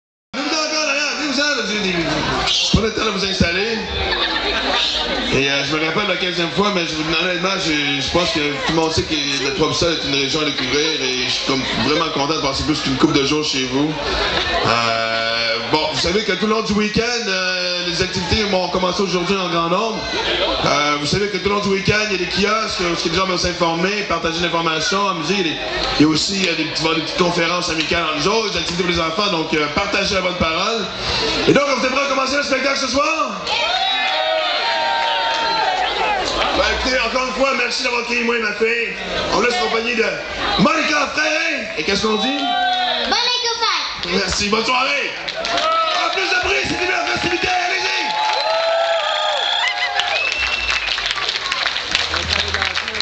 Échofête_2008_Ouverture par Maxim Martin
vendredi 25 juillet, Discours d'ouverture de Maxim Martin, Président d'honneur